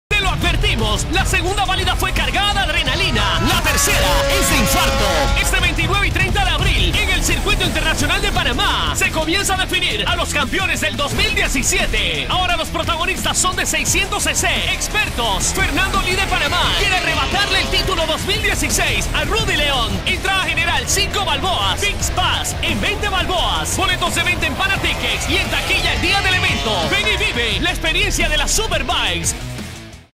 PREPARATE PARA ESTE 29 Y 30 TERCERA VALIDA DE SUPER BIKE EN EL CIP. TODABIA ESTAS A TIEMPO DE FORMAR
PREPARATE PARA ESTE 29 Y 30 TERCERA VALIDA DE SUPER BIKE EN EL CIP. TODABIA ESTAS A TIEMPO DE FORMAR PARTE DE LOS PILOTOS QUE CORRERÁN ESE DIA, PARA MAYOR INFO. Oliver Mario Forero